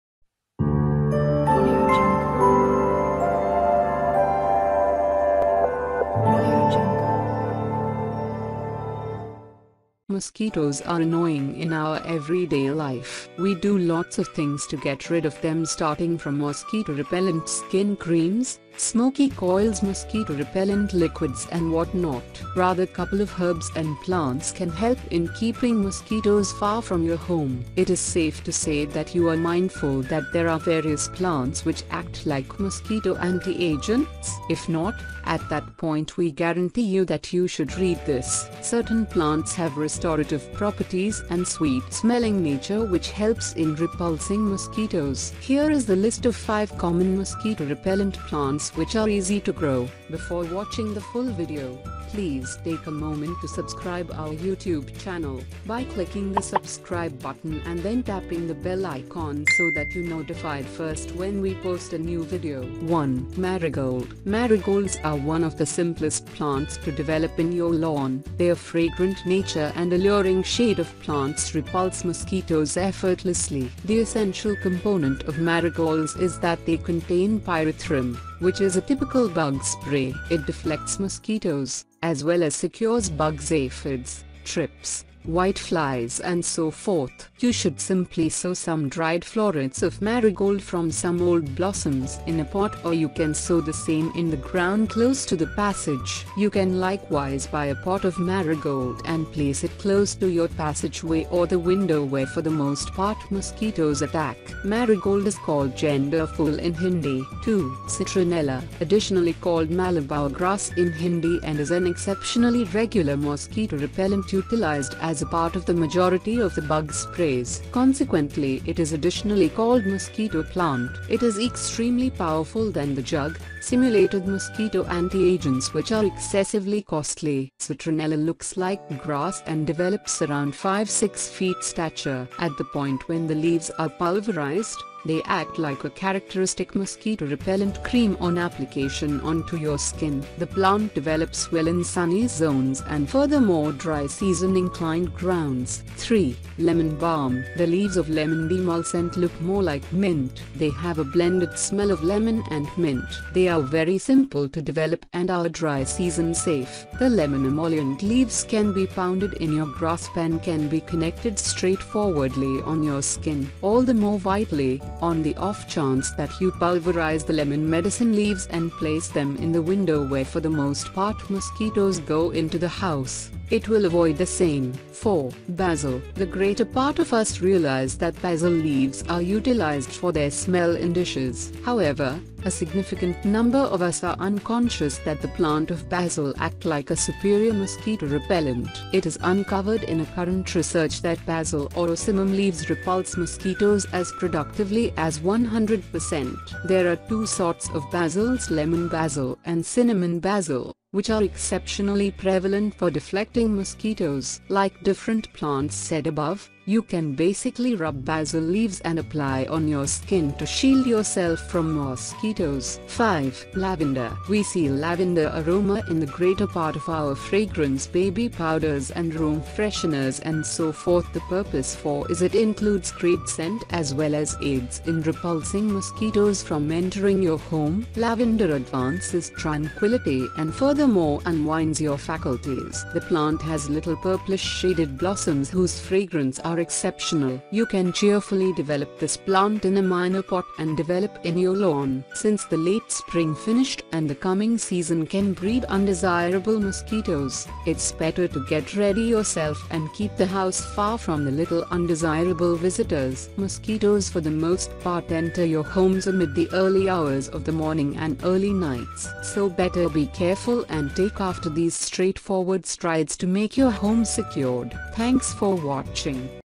5 Mosquito Repellent Plants and How to Use Them
5 Mosquito Repellent Plants and How to Use Them

Mosquitoes are annoying in our everyday life. We do lots of things to get rid of them starting from mosquito repellent skin creams, smoky coils mosquito repellent liquids and what not. Rather couple of herbs and plants can help in keeping mosquitoes far from your home. It is safe to say that you are mindful that there are various plants which act like mosquito anti-agents? If not, at that point we guarantee you that you should read this. Certain plants have restorative properties and sweet-smelling nature which helps in repulsing mosquitoes.

Here Is The List Of 5 Common Mosquito Repellent Plants Which Are Easy To Grow.

1. Marigold.
2. Citronella.
3. Lemon Balm.
4. Basil.
5. Lavender.